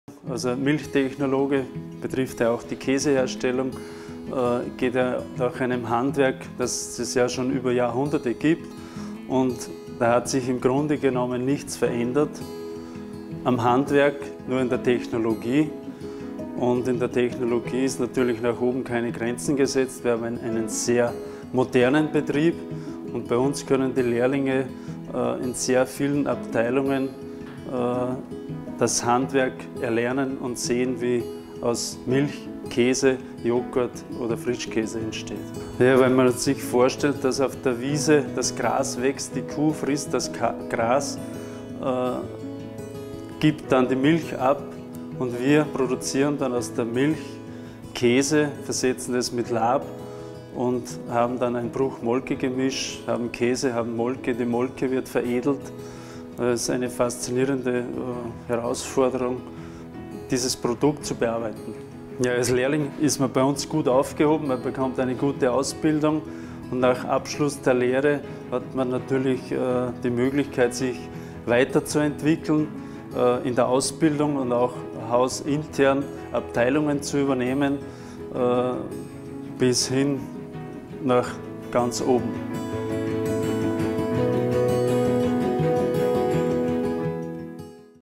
Lehre bei der SalzburgMilch: Milch- & Lebensmitteltechnologie in der Käserei der SalzburgMilch
Von der Übernahme der Rohmilch bis hin zur Veredelung zu köstlichen Käsesorten und deren
sorgfältiger Qualitätsprüfung: Milch- und Lebensmitteltechnologen erwarten viele spannende
Tätigkeitsfelder. Jetzt mehr erfahren!